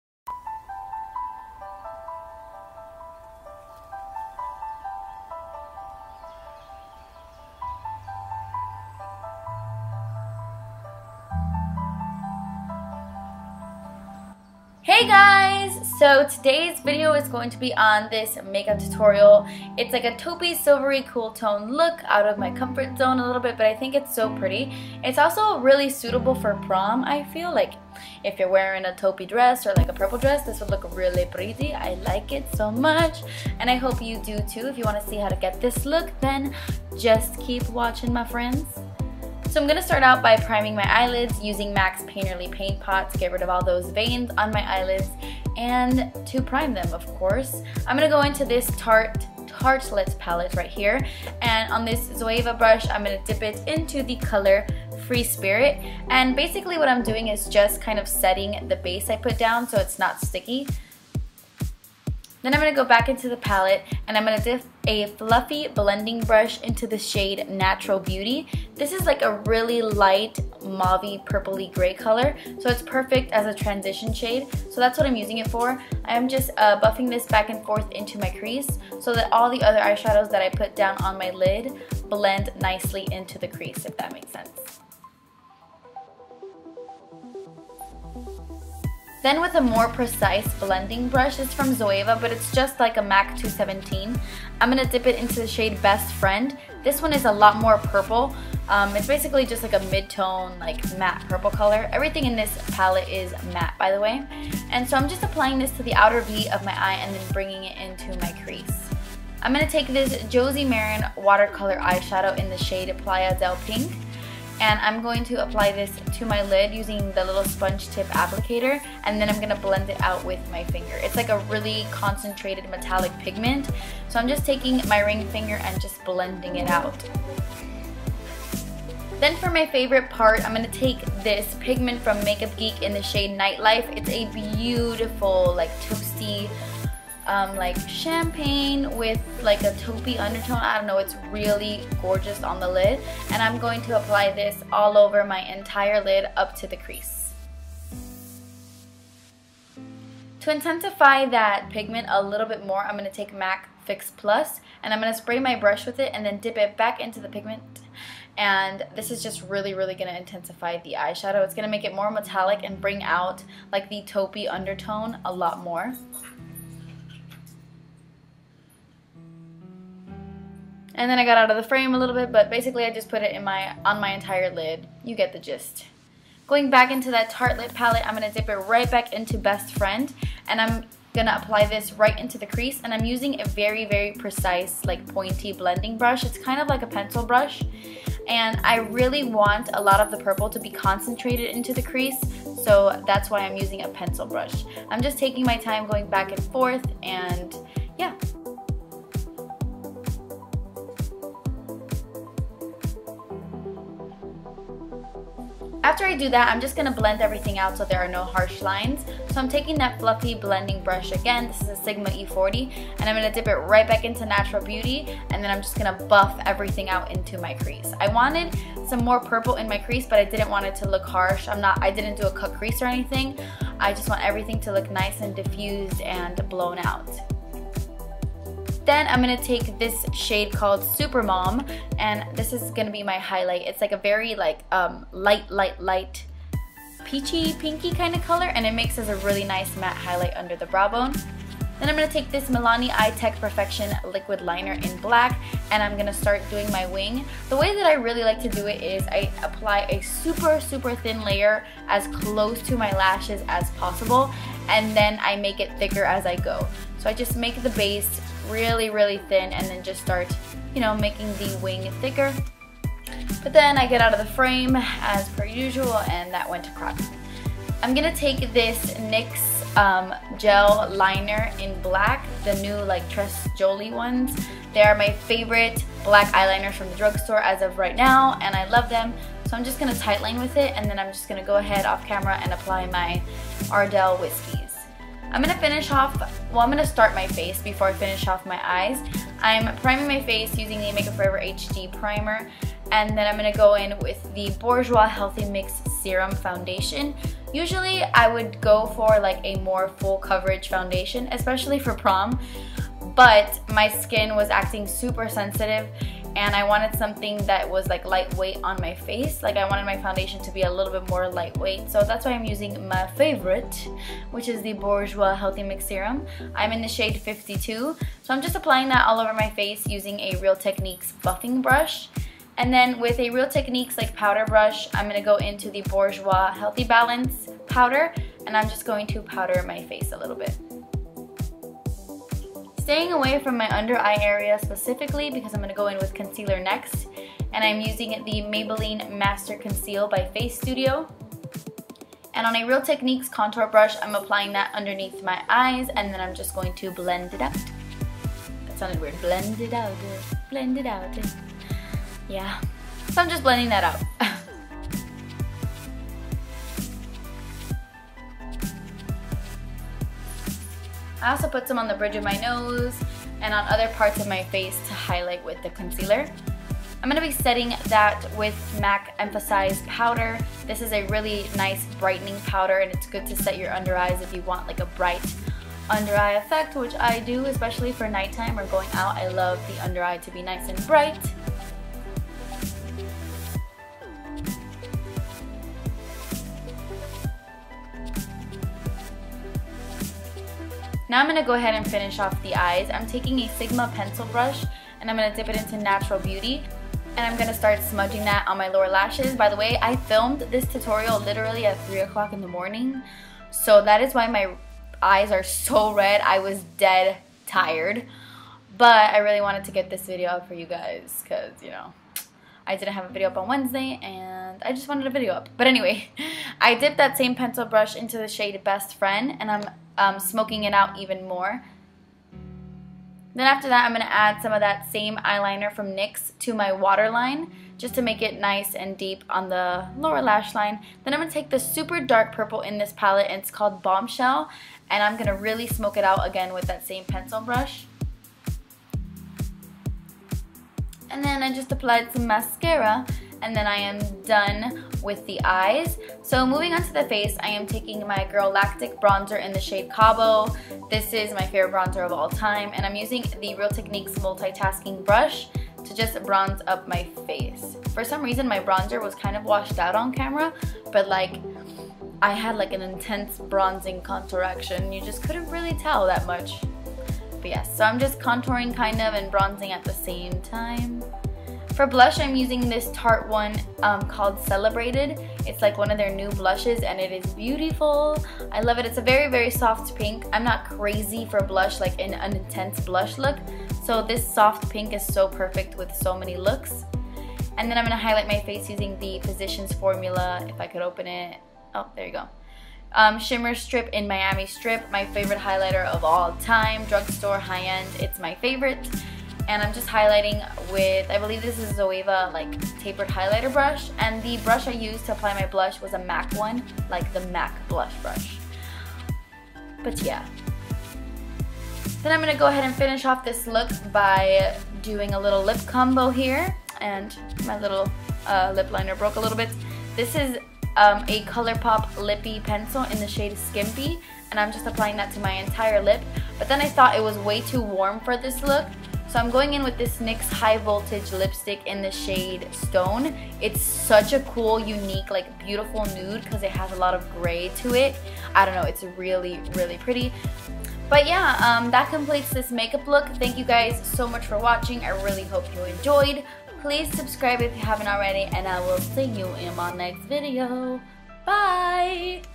Taupe & Purple Makeup Tutorial
Hey, Guys!
Here is a makeup tutorial for you! I hope you enjoy!

Products I Used:
Tarte Tartelette Palette
Josie Maran Water Color Eyeshadows in Playa Del Pink
Makeup Geek Pigment in Nightlife
Nyx Tres Jolie Gel Liner in Black
Ardel Wispies False Lashes
Jordana Best Lash Extreme
MUFE HD Primer
Bourjois Healthy Mix Serum in 52
Bourjois Healthy Balance Powder in 53
Girlactik Cabo Bronzer
Tarte Amazonian Clay Blush in Celebrated
Physicians Formula Shimmer Strip in Miami Strip
Colourpop Lippie Pencil in Skimpy
Nix High Voltage Lipstick in Stone

What I’m Wearing:
Earrings: Forever 21
Top: Forever 21
Nails: Julie G “Cabana Boy”
Brows: Anastasia Brow Wiz in Dark Brown and Mac Cork Eyeshadow

—FAQ:
*Where did you get the owl on your desk?
Michaels
*Where did you get your vanity?
Danny actually built it for me